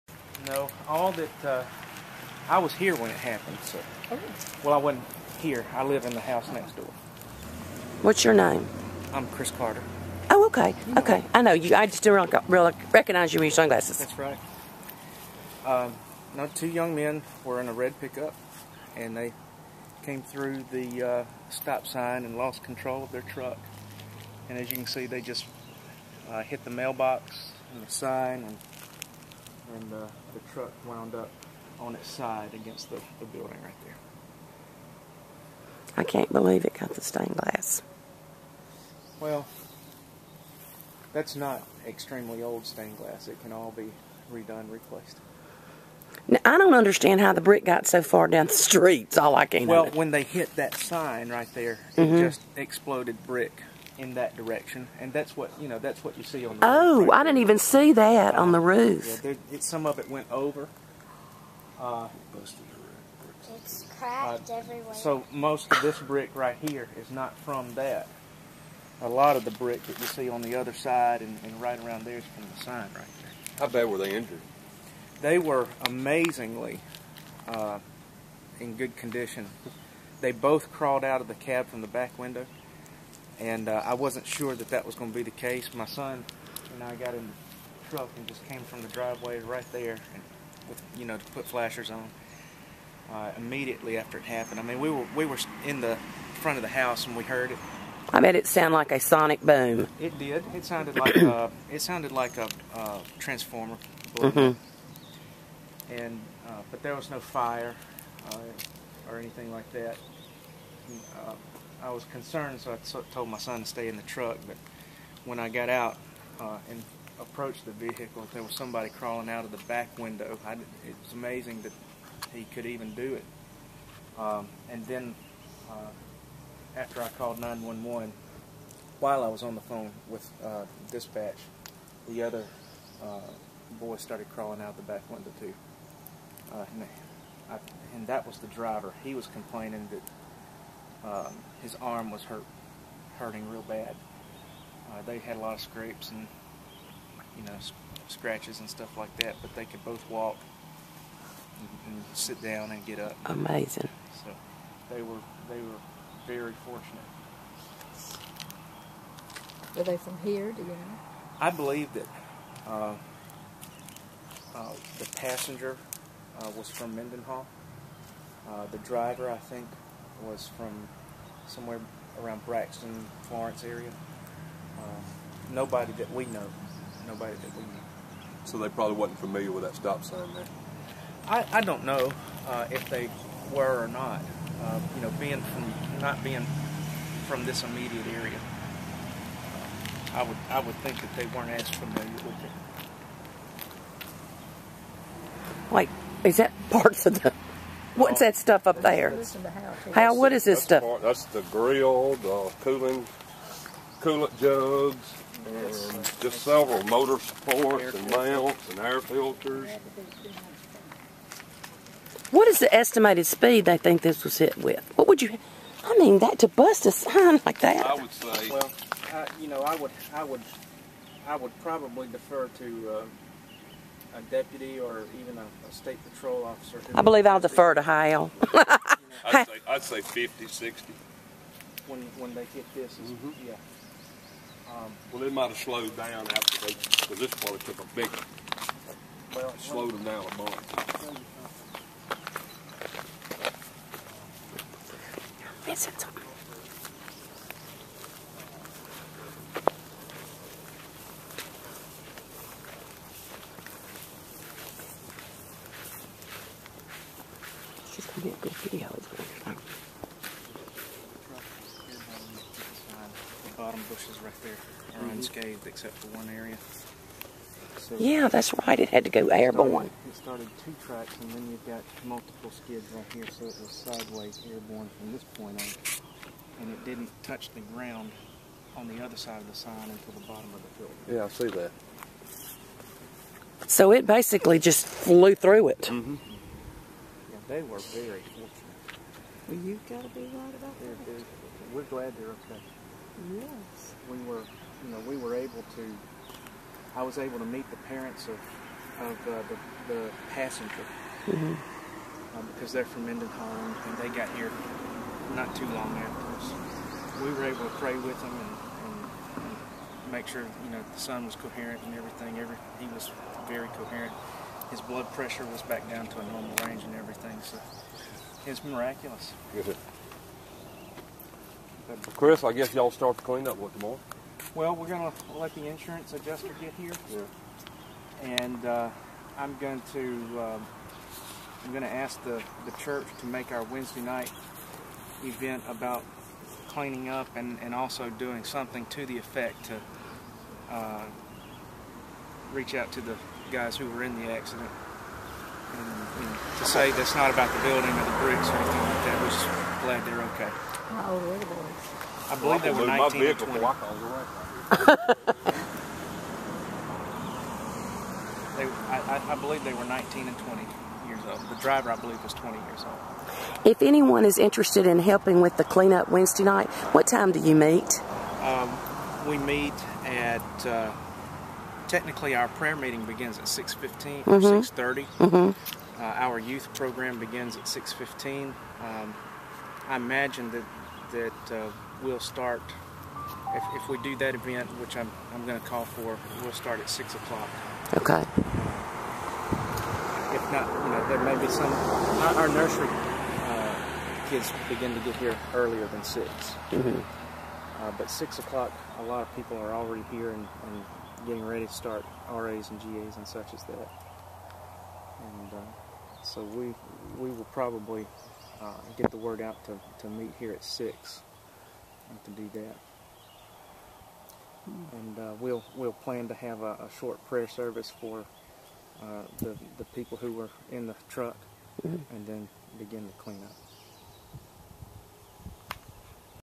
Goodwater Baptist Church hit by truck
Truck goes airborne and hits church sign then slams into church